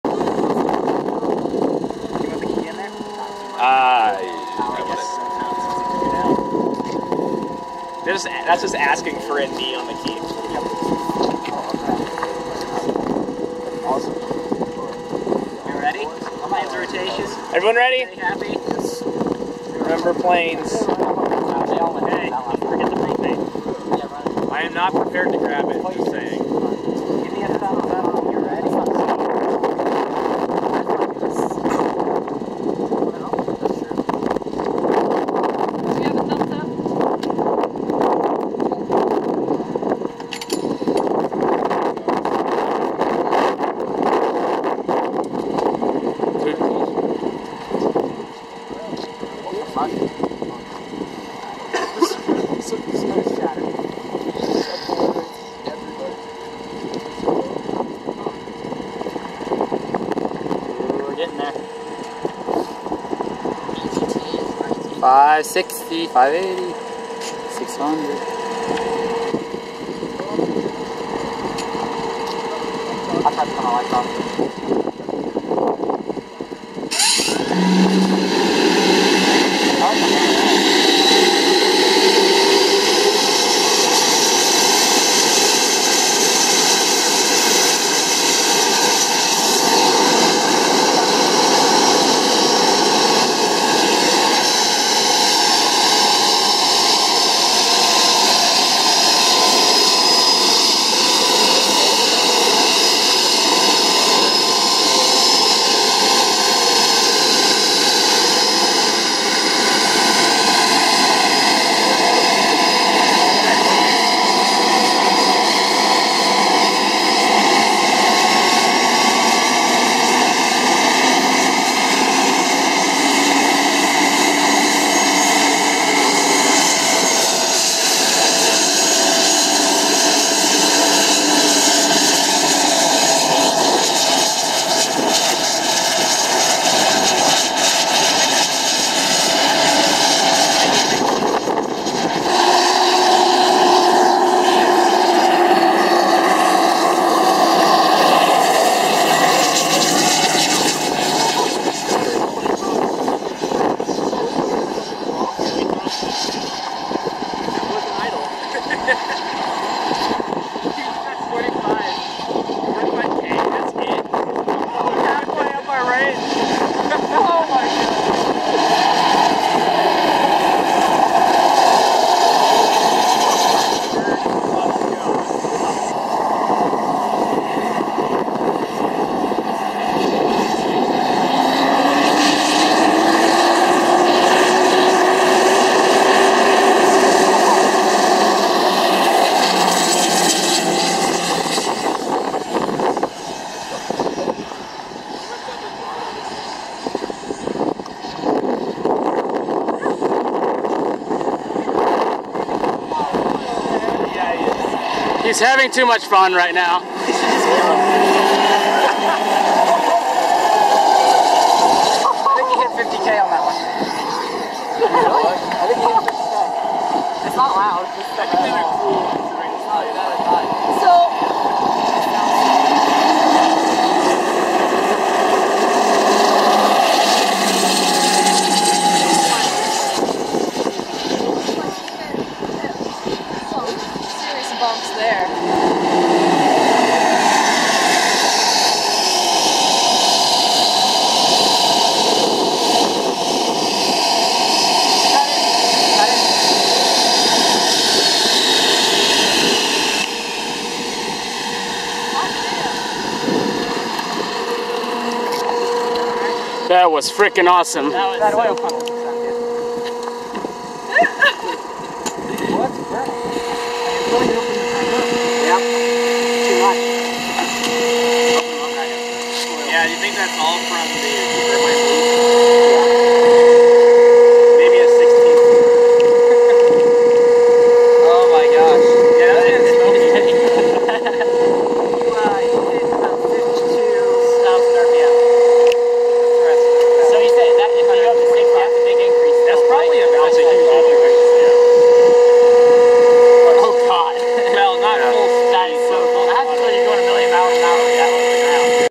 Jet Kart Turboshaft Second Higher Power Run B roll ft. HighVoltageFeathers
Second run of the jet kart turboshaft vehicle. This time, the taps were opened a little bit.